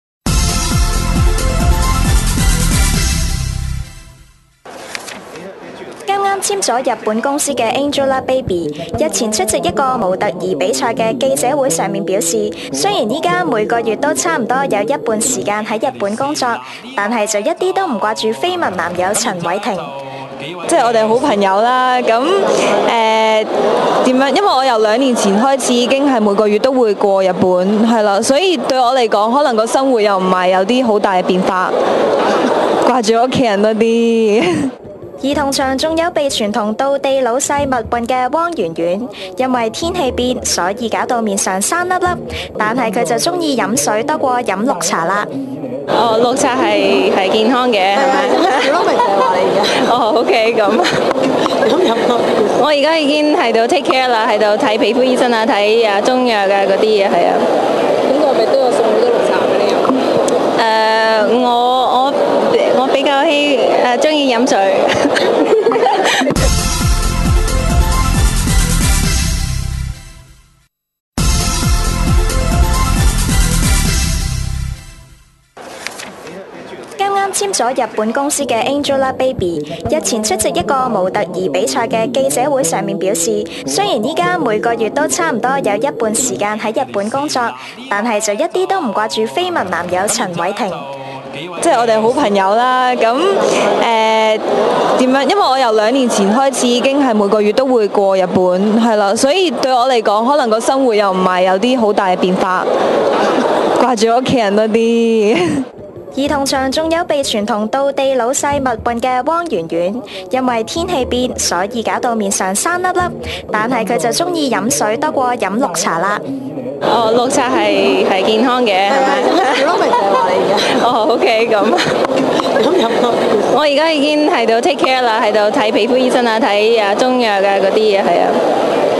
Popular William Chan Wai-Ting & Angelababy videos
Popular William Chan Wai-Ting & Angelababy videos Angelababy前晚出席「海港城STYLE Model Hunt模特兒大賽」記者會，剛簽約日本公司Avex的她稱，公司會幫她接洽日本方面的工作，同樣由模特兒工作開始，.

angelababy.janice man.